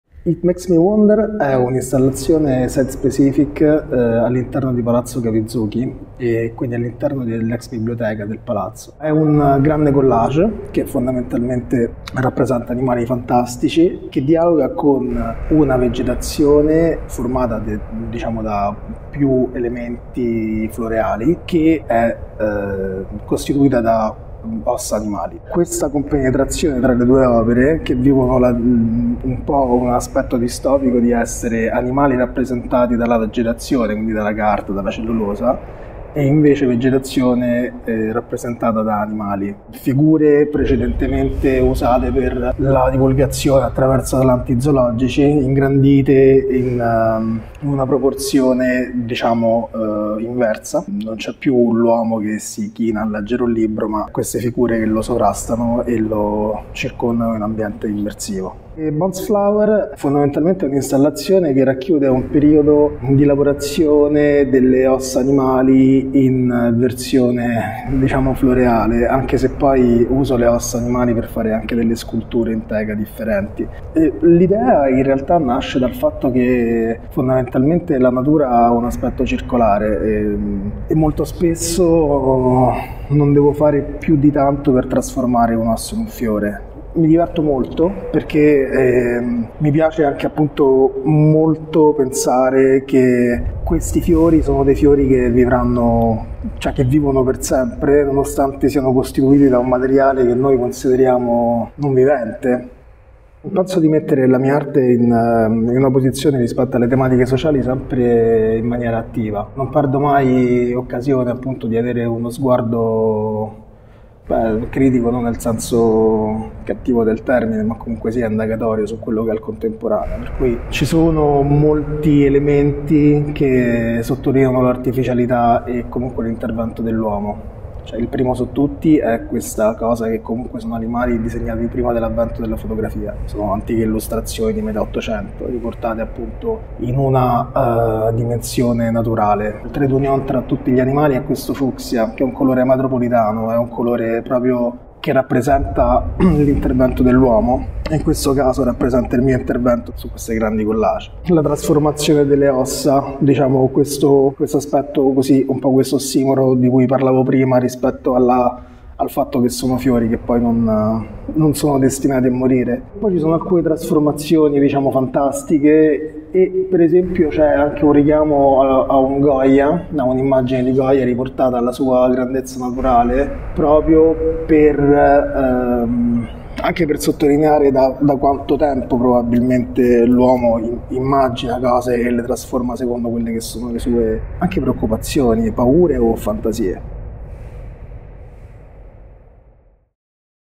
BANKERI: Wunderkammer di animali fantastici nella mostra Artificialis
Incontro con l’artista Bankeri e le sue opere pop e dissacranti, dove elementi fantastici e riflessivi si intrecciano in una nuova visione all’interno della mostra Artificialis
Curato da Valeria Picchi per Revenews.it